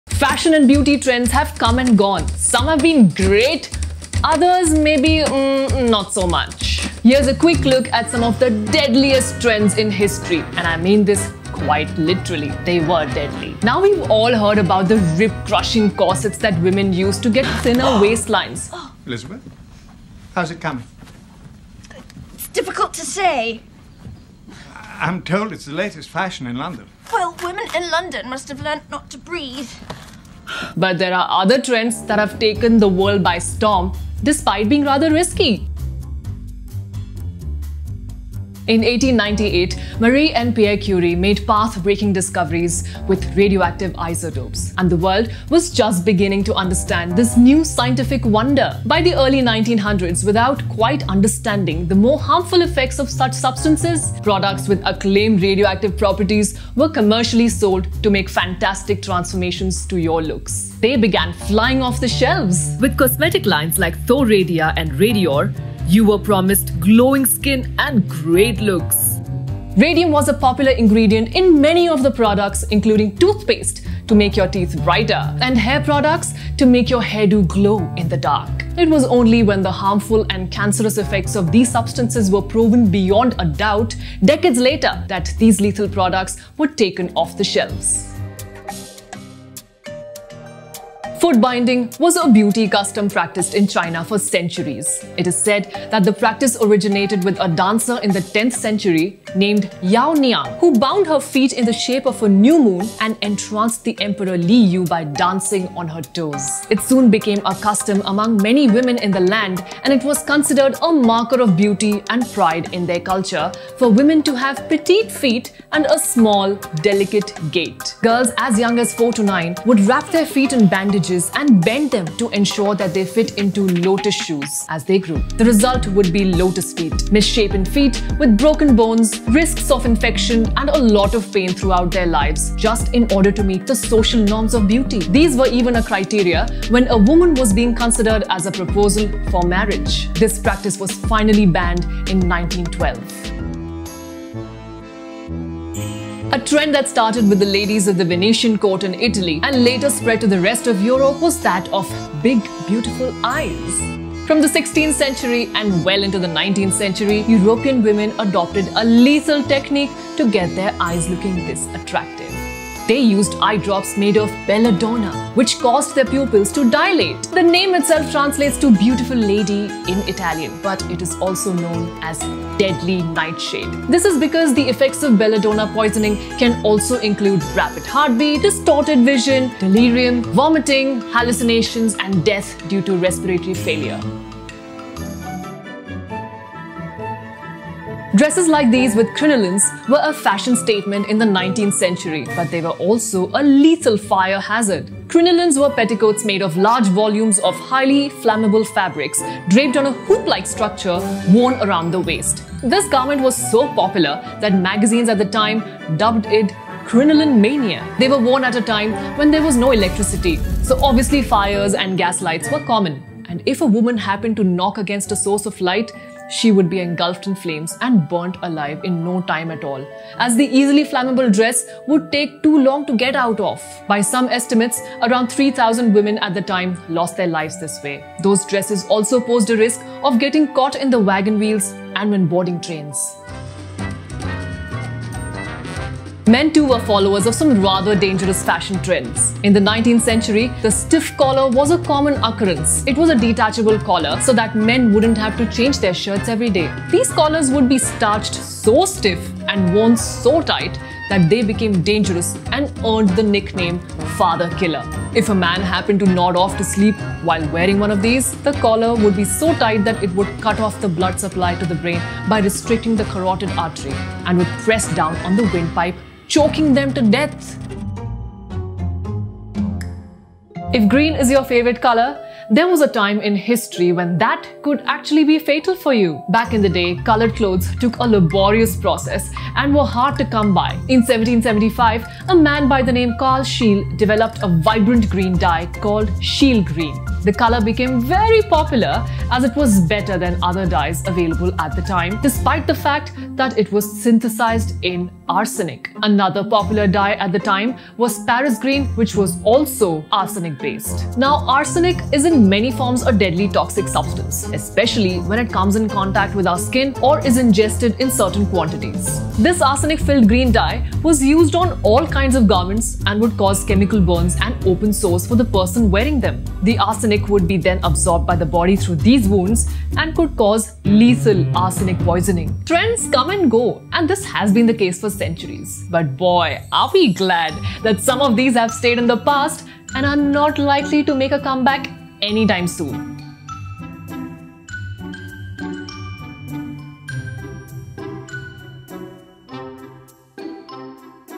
Most dangerous fashion trends in history | Nutshell
How far are you willing to go for fashion? Here are some lethal fashion trends from history that would literally SLAY!☠️

📍 VIDEO MAP
Intro 00:00
The rib crushing corsets 00:15
Radioactive beauty 00:38
Lotus feet 01:29
Lethal eyes 02:24
Lit dresses 03:06
Killer collars 03:54
Deadly dyes 04:29

CREDITS

Producer
Nakul Santpurkar

Writer
Chiara Saldanha

Director
Chiara Saldanha

Cast
Pooja Bhamrrah

Editor
Chiara Saldanha

Executive Producer
Jinal Mandot

Design and Animation
Aghil Prasannan

Sound Design
Hardik Desai

Casting
Clout

Casting Associate
Ashish Chawla

Video Operations
Lavanya Rakesh

Subtitle
Sharmin Vohra